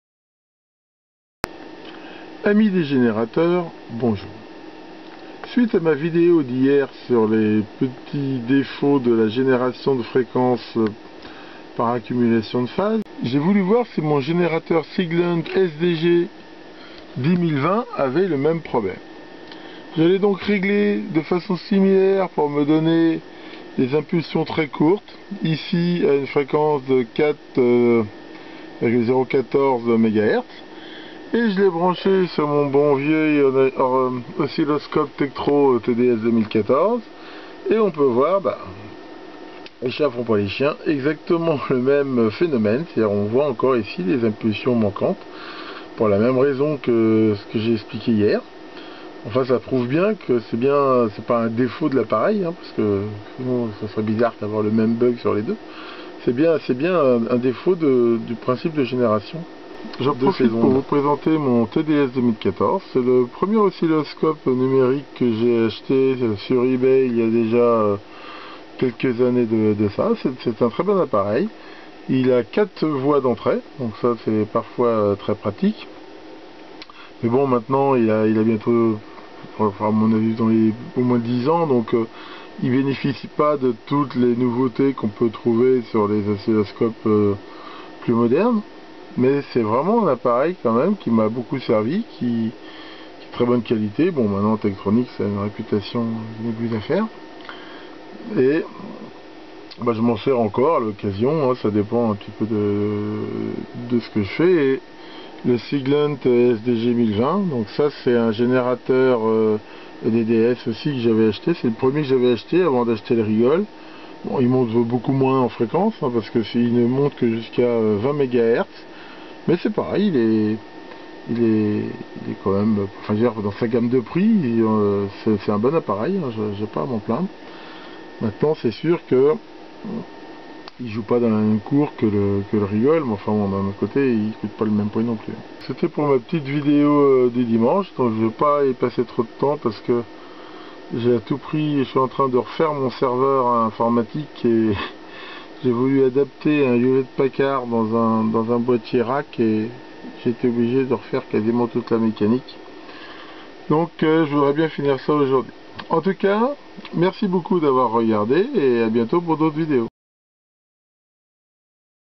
Cyrob: Limites DDS, essais sur le Siglent SDG1020
Suite à ma vidéo d'hier sur les grandeurs et décadences de l'accumulation de phase, j'ai voulu faire la même manip avec mon autre géné DDS et mon bon vieux Oscillo Tektronix TDS2014. Même motifs, même punitions...comme prévu, on observe le même phénomène.